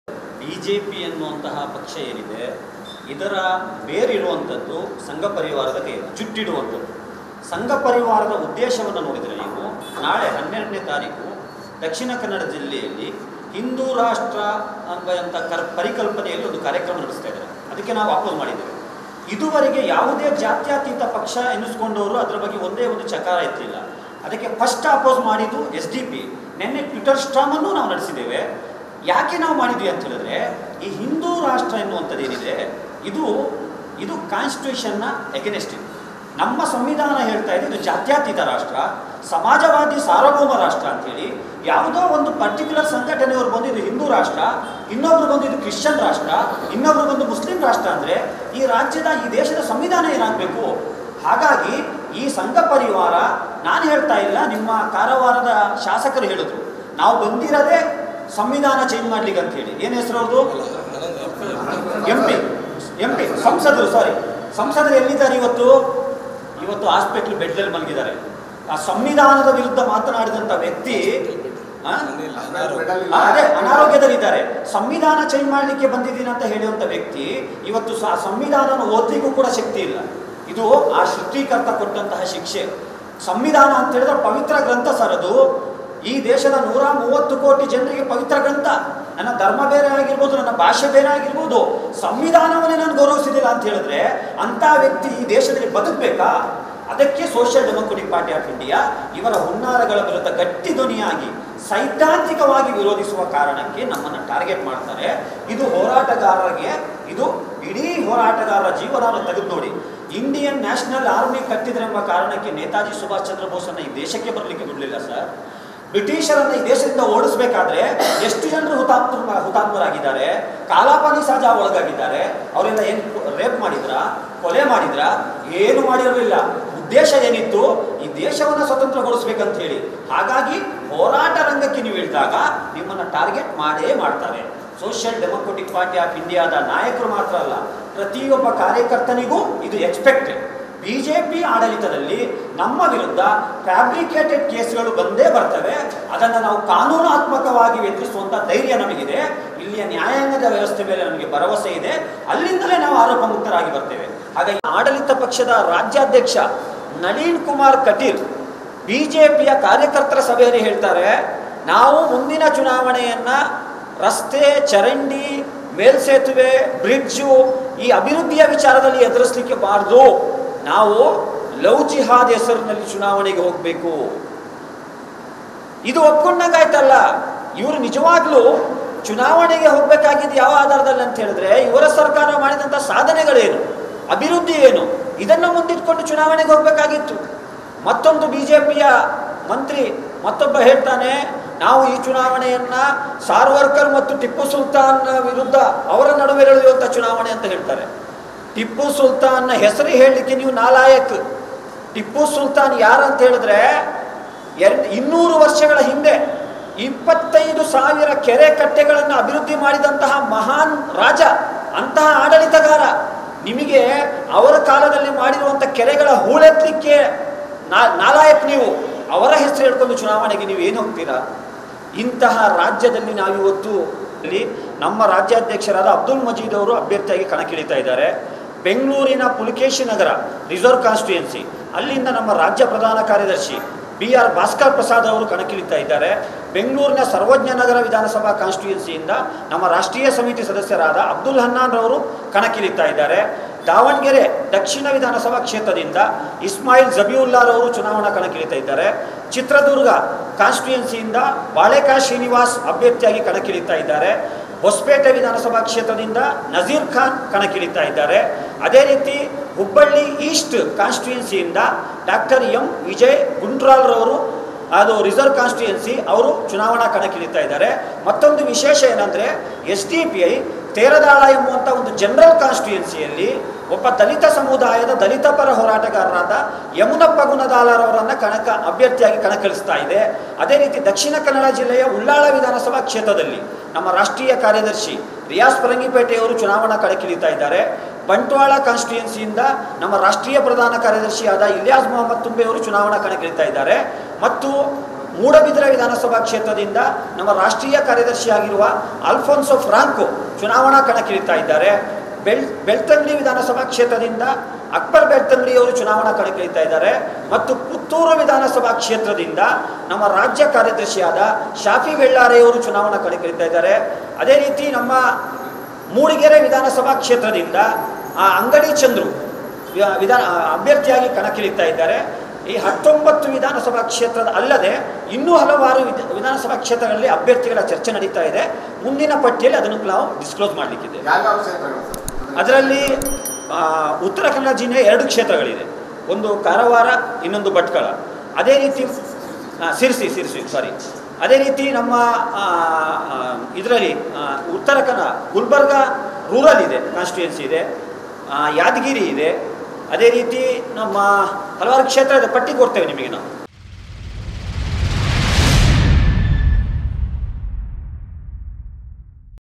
Afsar Kodlipet: ಸಂವಿಧಾನ ಚೇಂಜ್ ಮಾಡ್ತೇನೆ ಅಂದವ್ರಿಗೆ ಇಂದು ಸಂವಿಧಾನ ಓದೋಕು ಶಕ್ತಿ ಇಲ್ಲ | Love Jihad | SDPI
In Bhatkal pressmeet SDPI worker Afsar Koldlipet said Where are the parliamentarians who talked about the constitution today? Uttara Kannada MP Ananthakumar Hegde said we should change the constitution. The man who spoke against the constitution is ill today. The person who said he will change the constitution does not even have the strength to read the constitution. This is the punishment given by the creator. This statement was given  by SDPI State General Secretary and Election Incharge Officer, Kodlipet. Constitution is the holy book for 130 crore people of this country. Should a person who said that he does not respect the constitution live in this country? SDPI has been targeted for strongly opposing his move. BJP's is in the hands of the Sangh Parivar. BJP is holding a program tomorrow in Dakshina Kannada under the concept of Hindu Rashtra, and we are against it. Those who claim to be a secular party have no qualms about it. Hindu Rashtra is against the constitution of the country. Some organization came and said this is Hindu Rashtra. If another comes and says this is a Christian country, then another will say it is Muslim country, what should be the constitution of the country, he added.